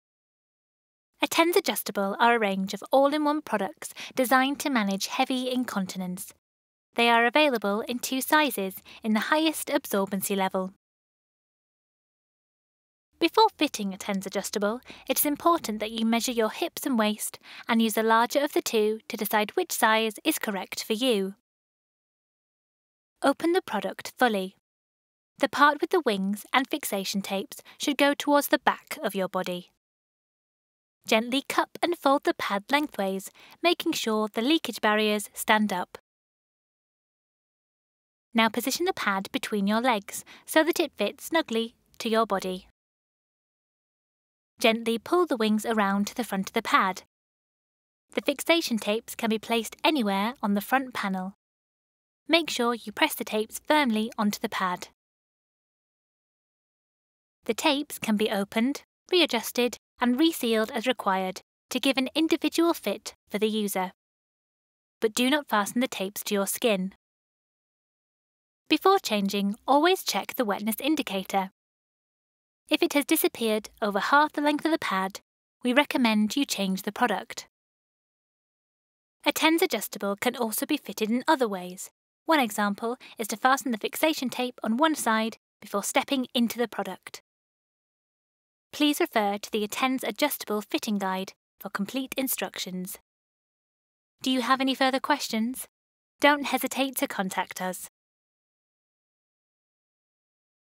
Attends Adjustable Fitting Guide
Attends Adjustable are an all-in-one product designed to manage heavy/severe urinary and/or faecal incontinence. This user guide gives you step by step instructions on how to best use the product.